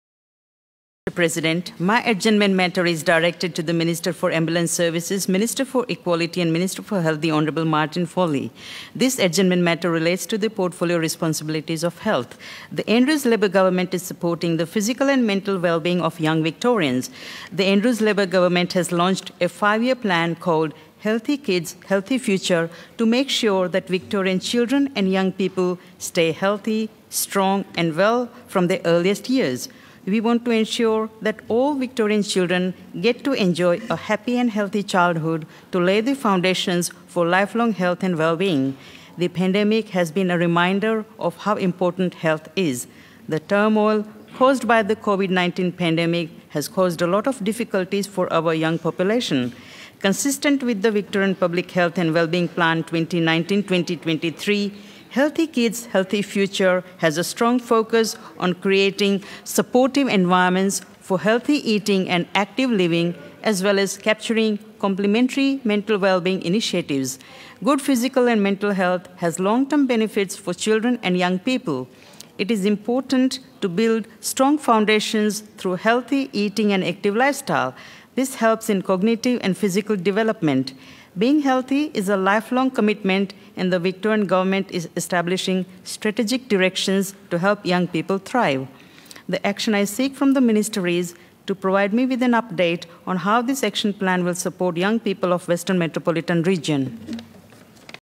Healthy Kids, Healthy Futures - Kaushaliya Vaghela MP
HEALTHY KIDS, HEALTHY FUTURES

The Victorian Government has launched a five-year plan called *Healthy Kids, Healthy Futures* to make sure that Victorian children and young people stay healthy, strong and well from their earliest years.

I asked the Minister for Ambulance Services, Minister for Equality and Minister for Health, the Honourable Martin Foley to provide me with an update on how this action plan will support the young people of Western Metropolitan Region.